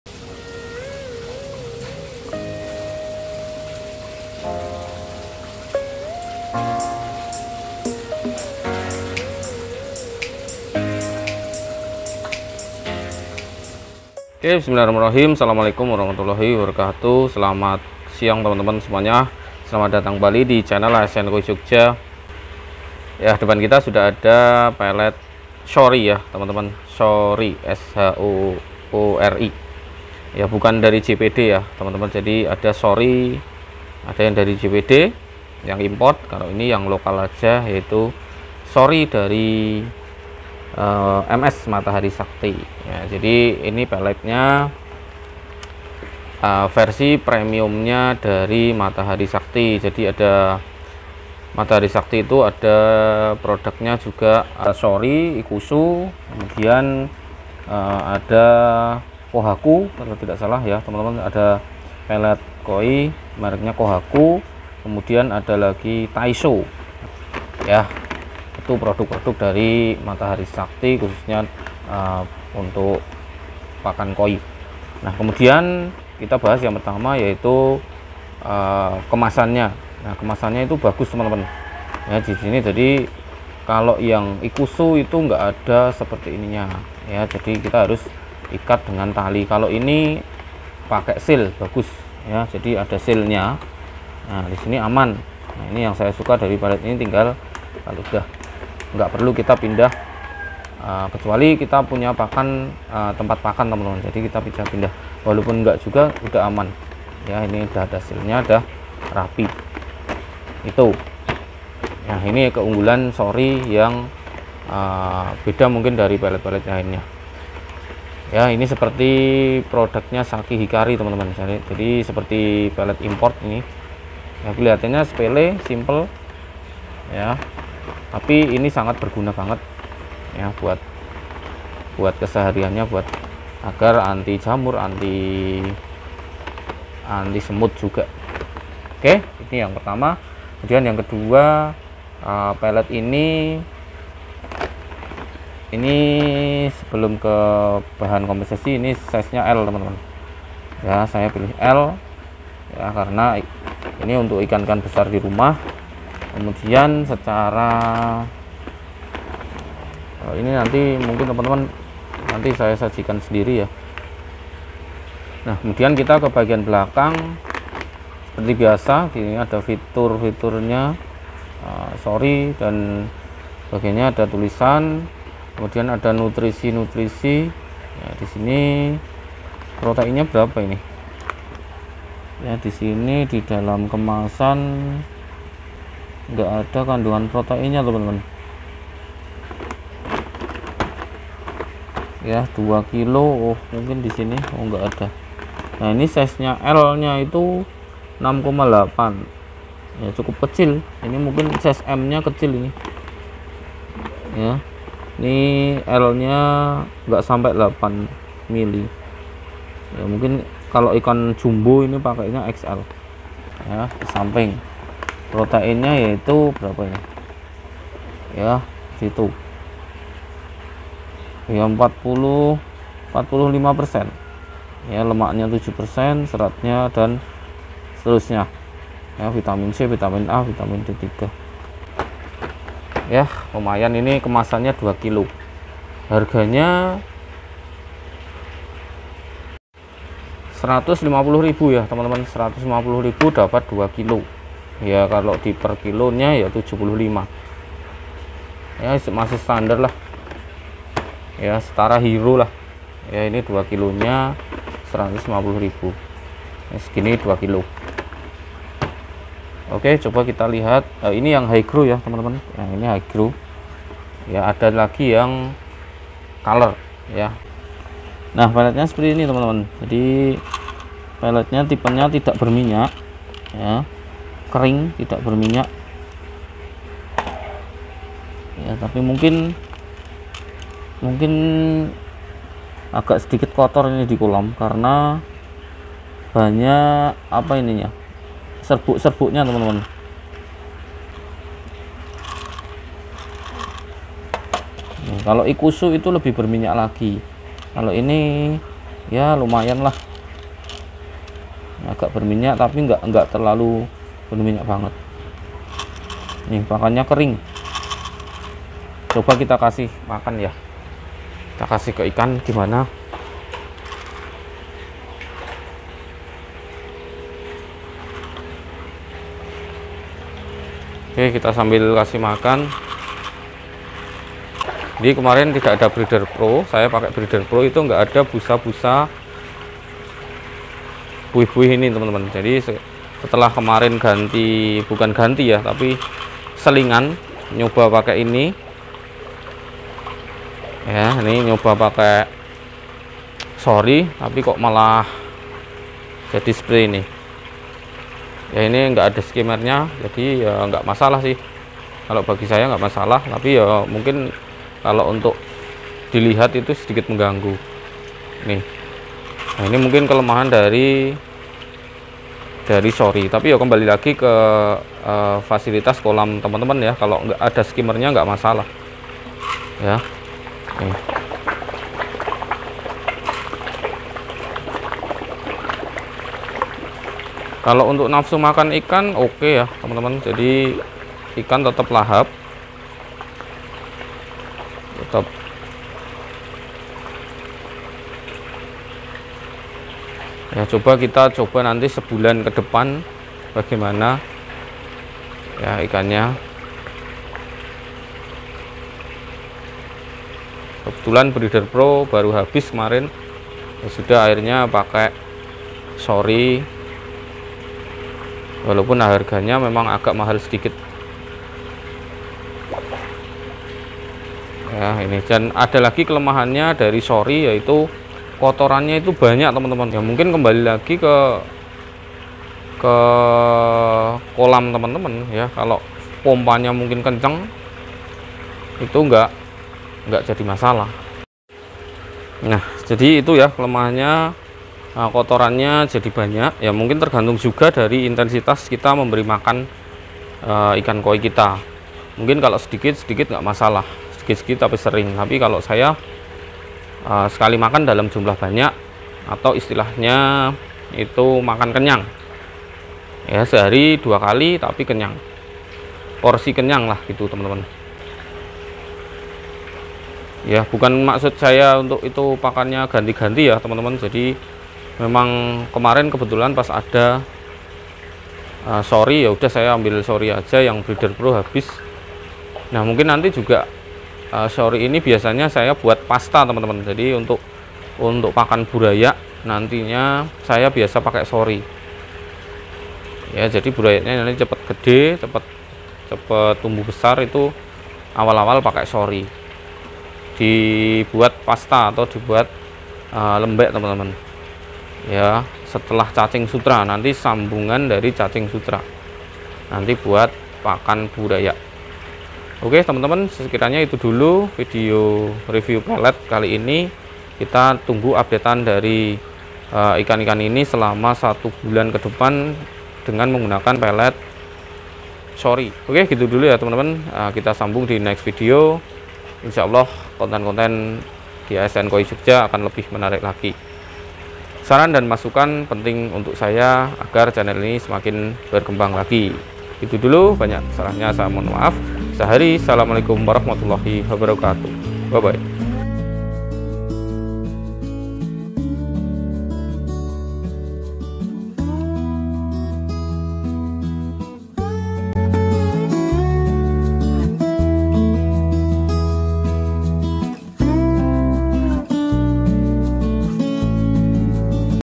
Pelet koi premium yang patut di coba | Pakan lokal rasa import ‼️
Shoori adalah pakan koi premium (superior) yang diformulasikan untuk ikan koi kelas impor maupun untuk koi kompetisi.